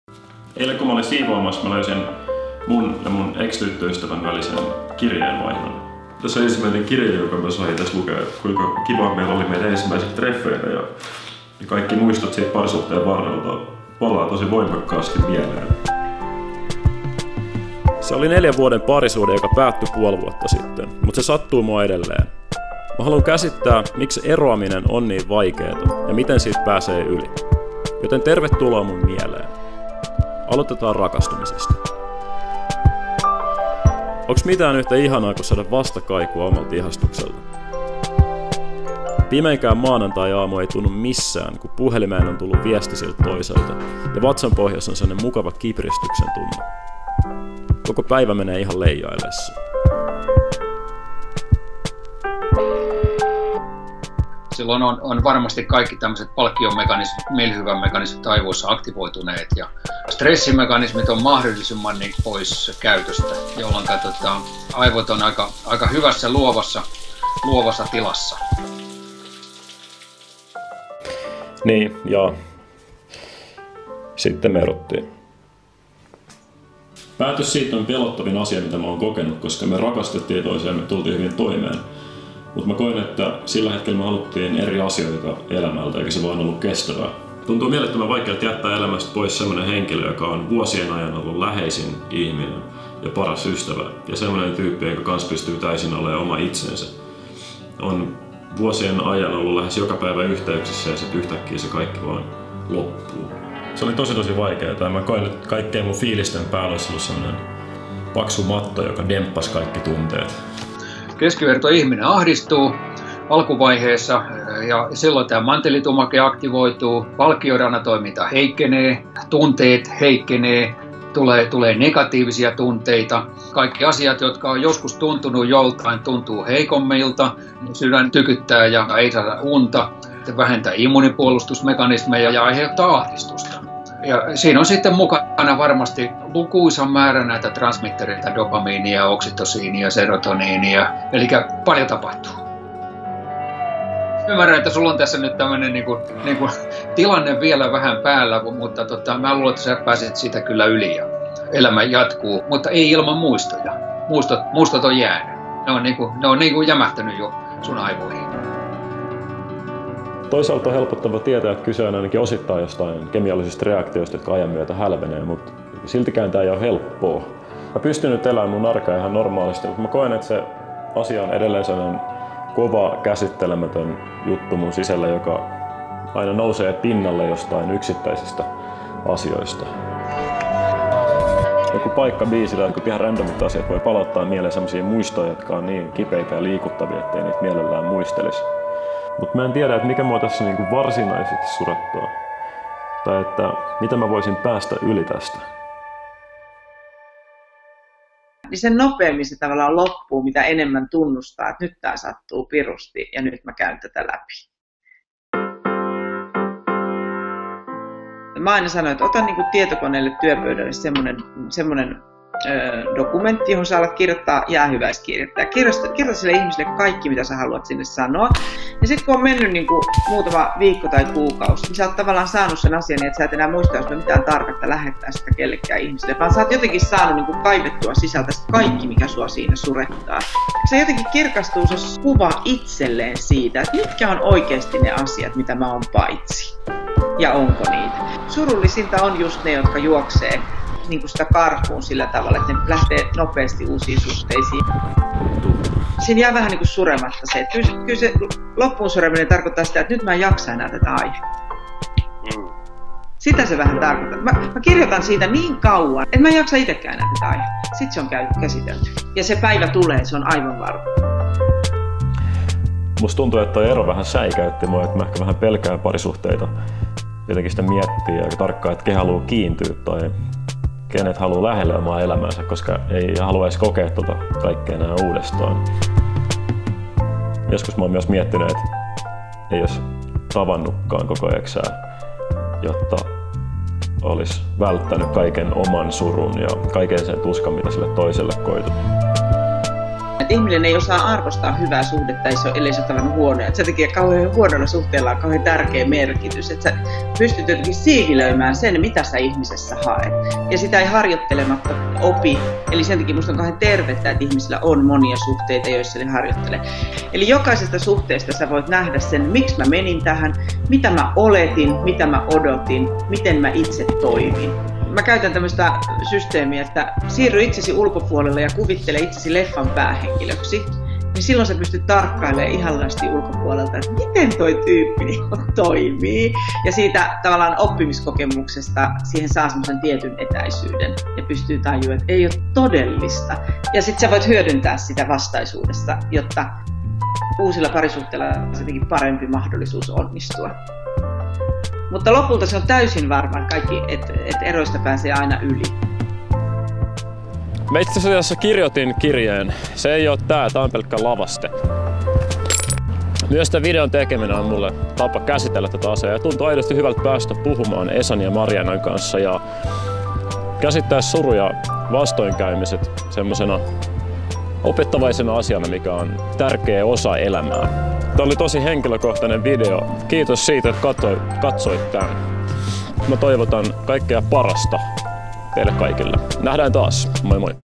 Kuinka päästä yli erosta – Jaakko Keso
Vaikeasti päättynyt parisuhde on ollut yksi vaikeimmista omista kokemuksistani. Koska en paini aiheen kanssa yksin, tein siitä videon.

Video- ja kuvalähteet:
Oma arkisto, Gettyimages, Google